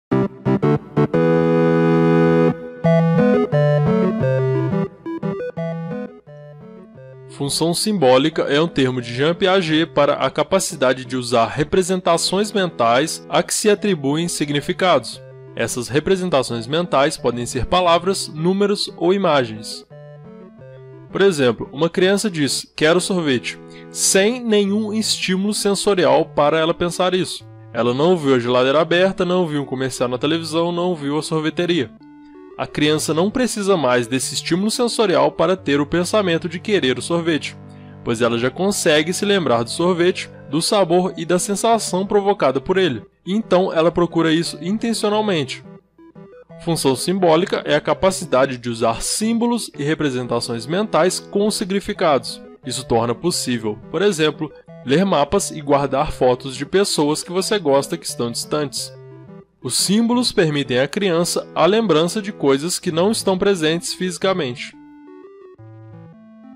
Função Simbólica - Piaget | Psicologia do Desenvolvimento
Entenda o que é Função Simbólica na teoria de Jean Piaget, um dos maiores nomes da Psicologia cognitiva e do Desenvolvimento.